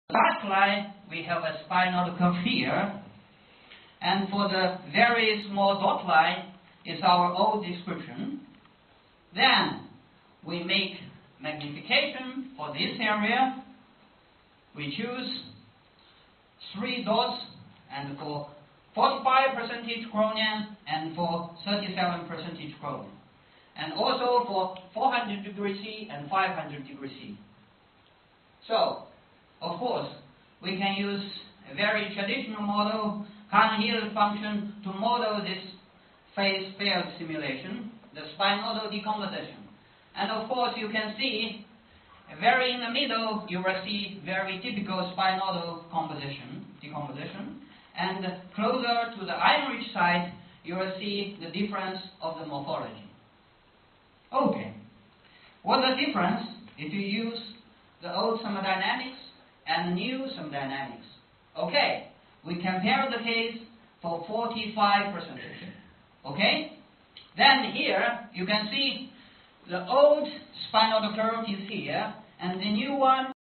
PTM2010 Presentation (1) by Wei Xiong - KTH
CALPHAD and phase field modeling, presentation in the PTM conference solid-solid phase transformations in inorganic materials, Palais des Papes, Avignon, France